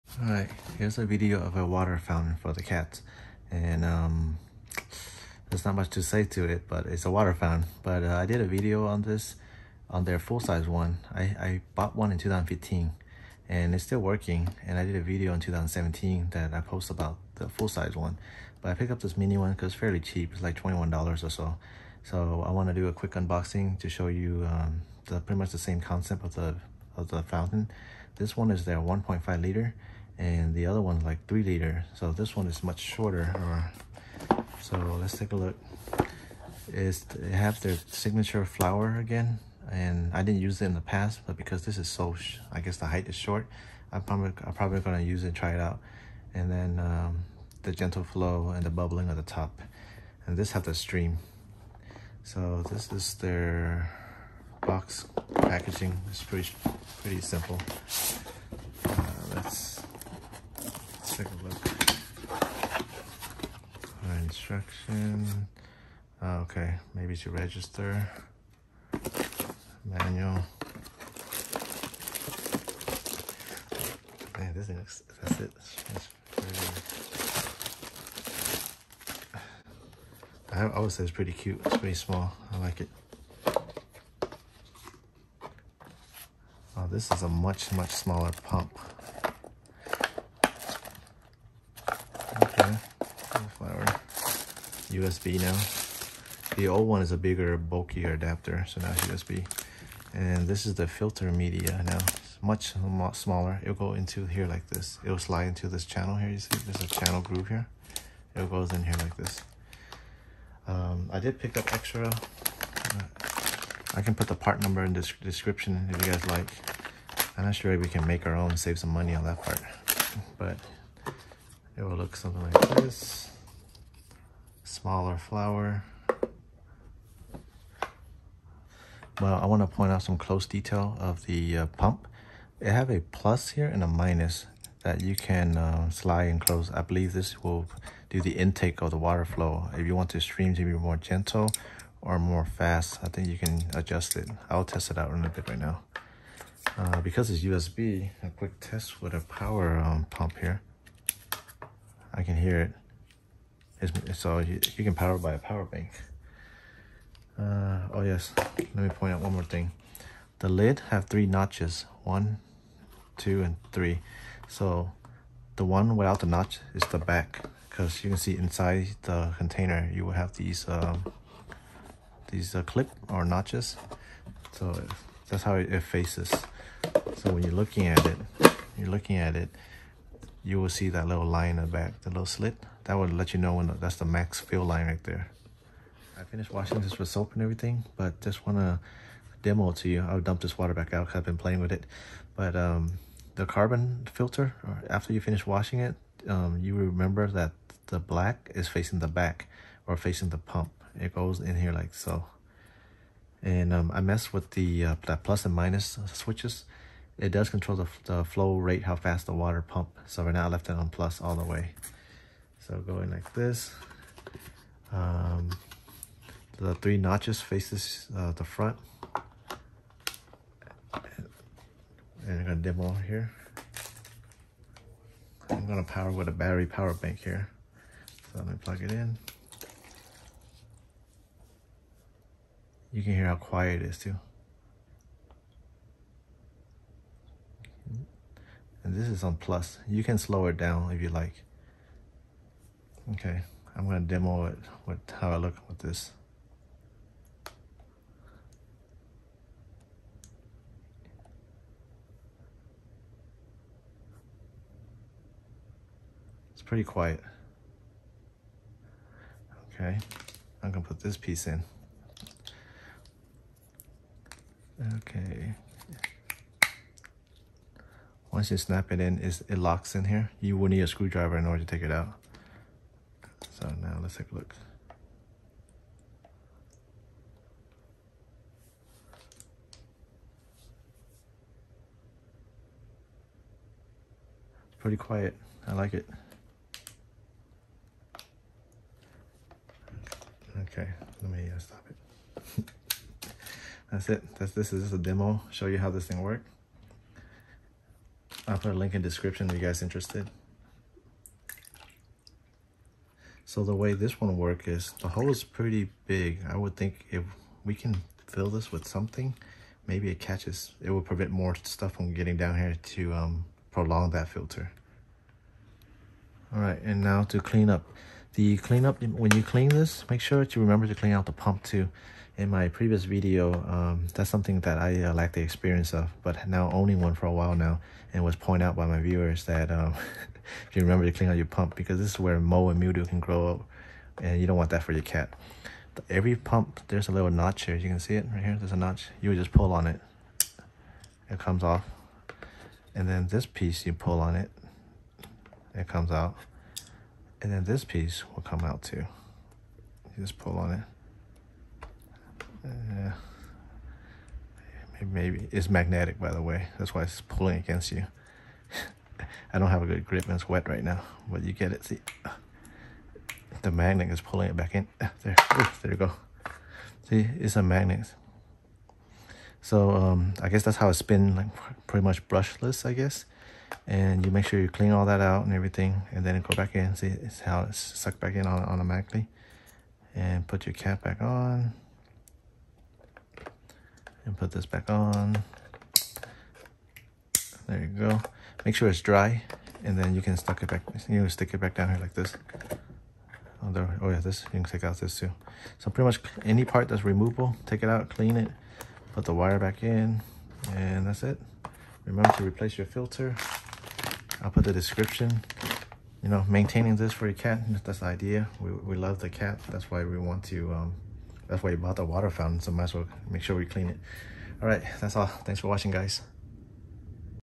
Catit Mini Flower Water Fountain Review
In this video, I will show you how to setup your mini Catit WaterFountain and now to clean and maintain it.

I just found out the brown box packaging is a toy for your cat. You can cut round circle holes and make it a treat box for your cat.

I have the full size one since 2015 and it still work after 5+ years! So remember to change out your filter every 30 days is key.

Catit Mini Flower Fountain

Catit Mini Fountain Replacement Filter